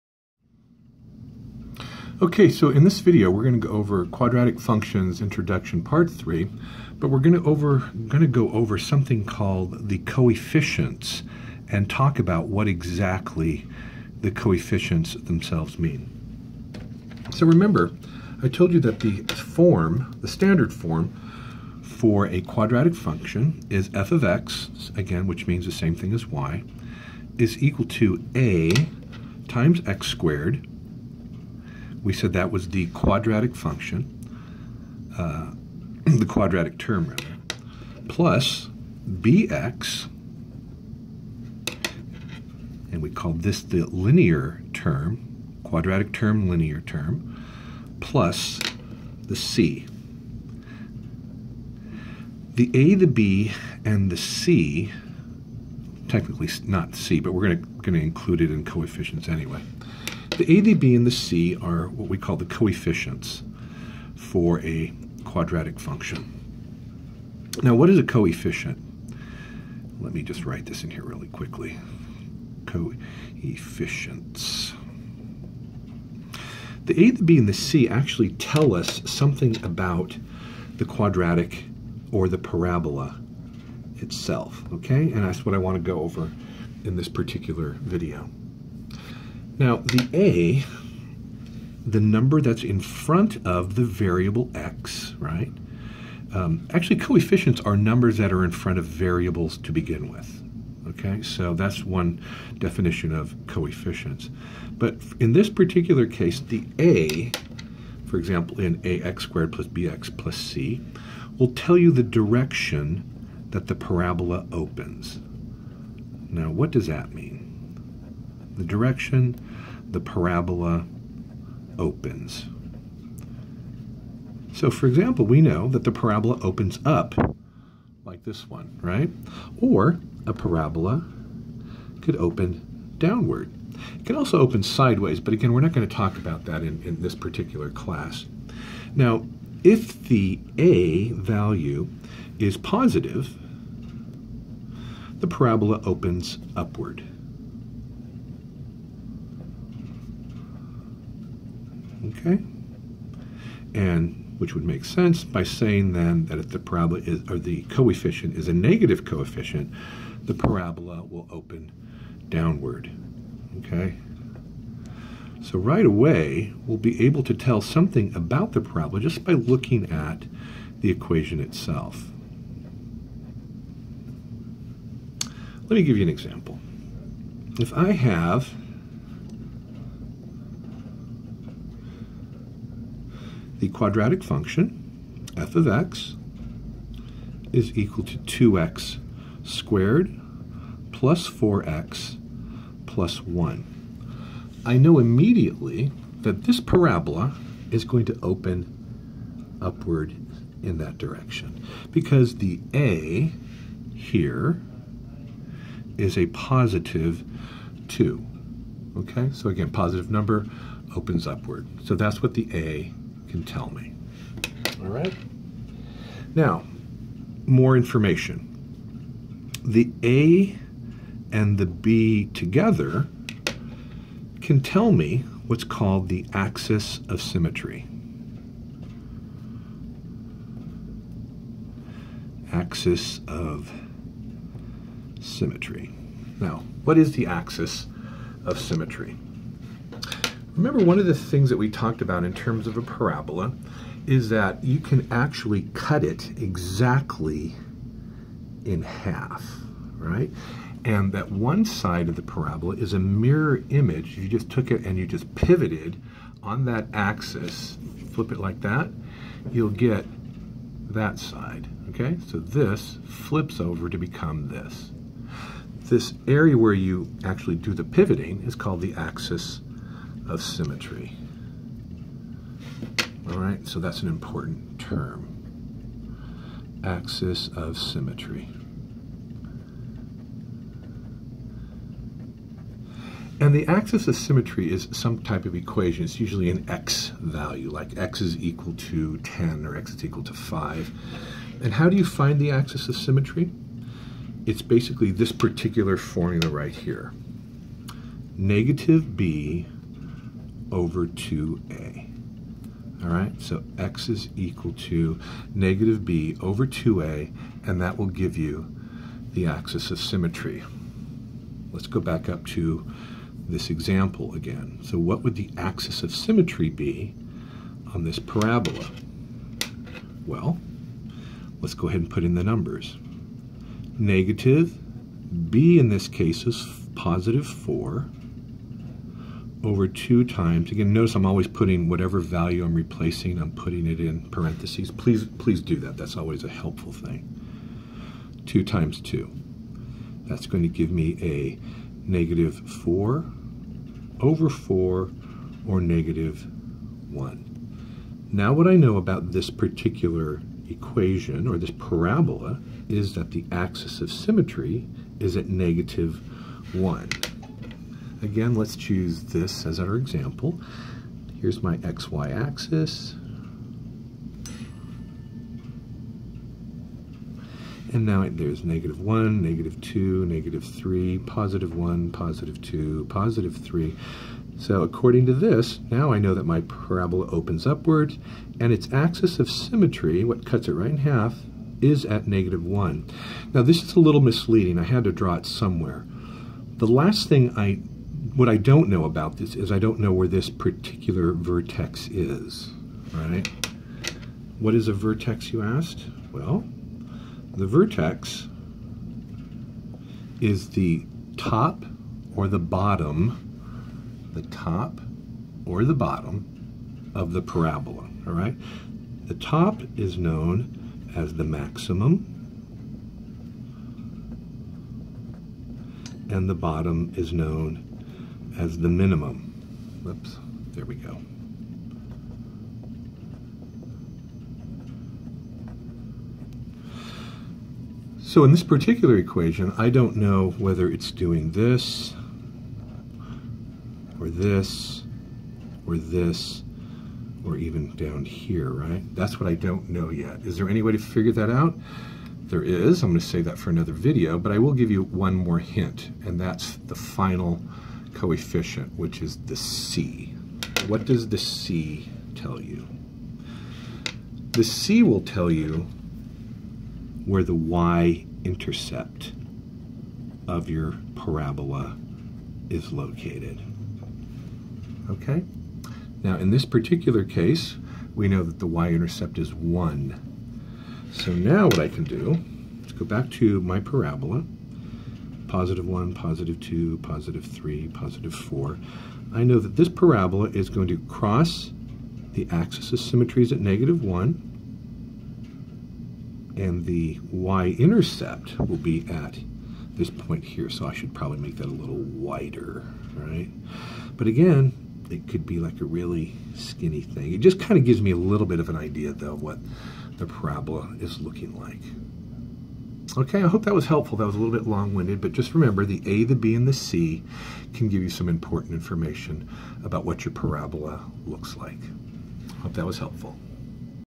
Quadratic Functions: Introduction (What do the Coefficients tell us?)
This video defines the meaning of coefficients in a quadratic function. The quadratic coefficient (a) describes which direction the parabola opens; the linear coefficient (b) in combination with the quadratic coefficient can be used to determine the equation for the axis of symmetry; and the constant (c) describes the y-intercept of the graphed parabola.